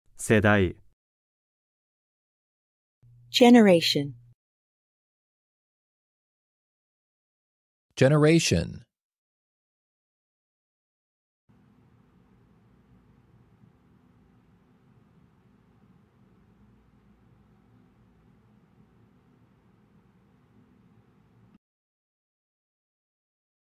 How to say "Generation" in Japanese
Male Voice:
世代

Female Voice:
世代

Male Voice Translation:
Generation

Female Voice Translation:
Generation